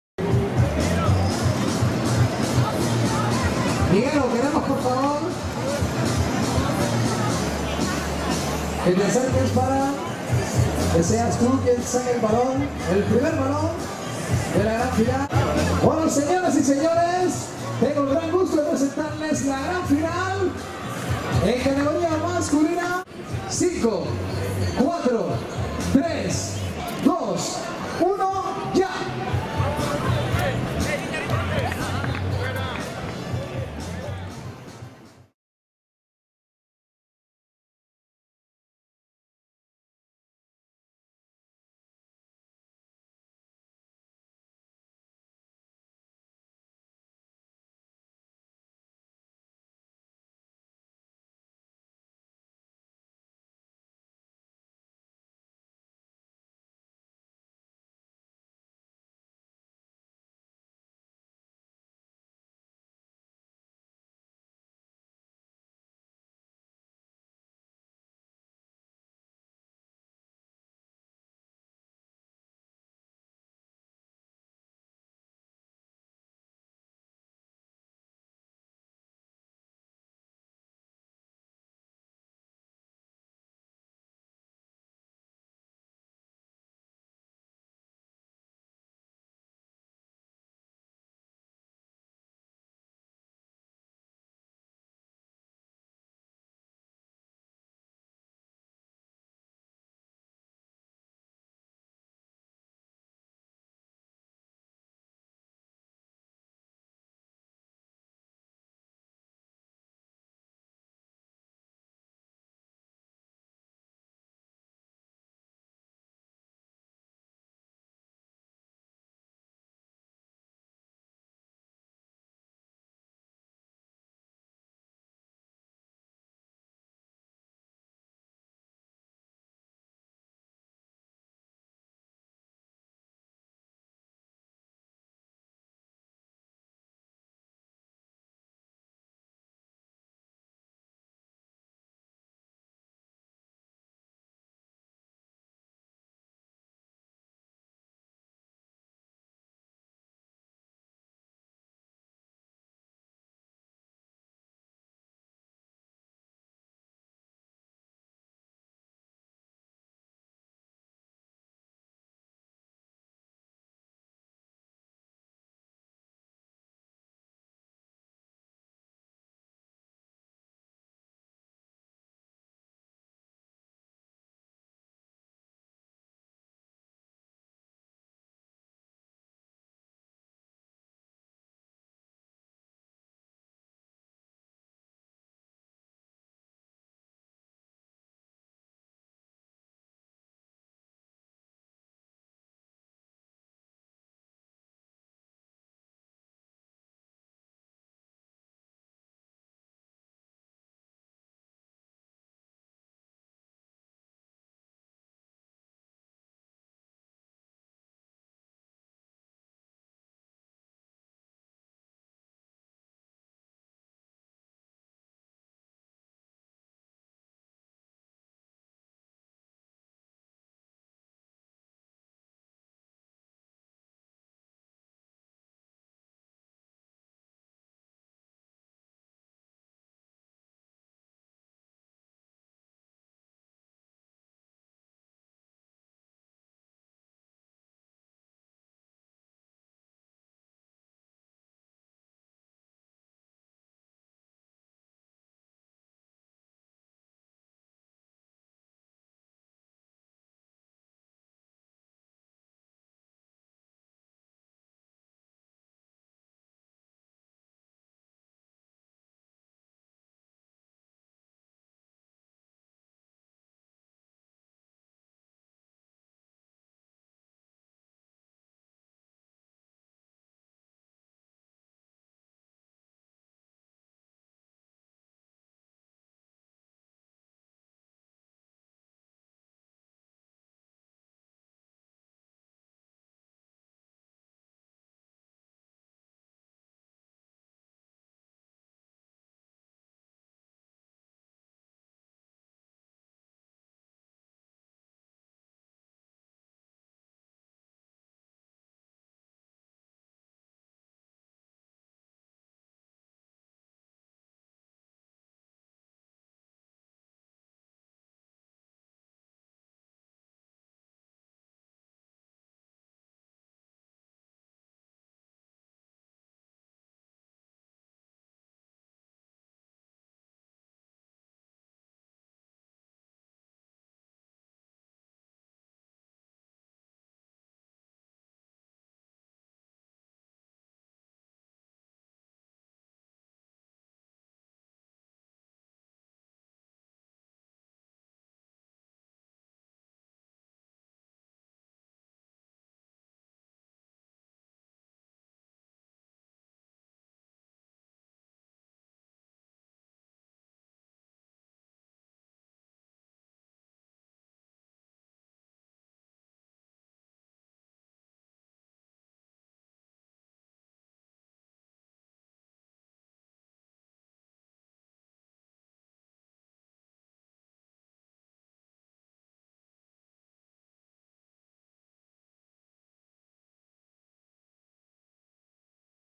2º Torneo Basket Playa - 2nd Beach Basketball Tournament – GRAN CANARIA vs AGÜIMES
05/02/2017 Las Palmas de Gran Canaria, Spain

Final Masculina GRAN CANARIA vs AGÜIMES 42-39 (Highlights)

Men's final GRAN CANARIA vs AGÜIMES 42-39 (Highlights)